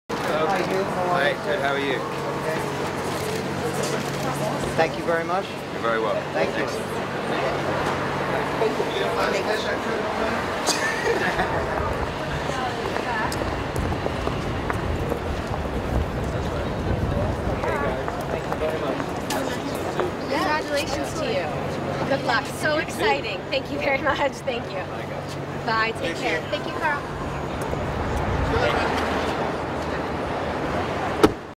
Hugh Dancy at the SiriusXM Radio studio Hugh Dancy at the...
Hugh Dancy at the SiriusXM Radio studio Hugh Dancy at the SiriusXM Radio studio on April 04, 2013 in New York, New York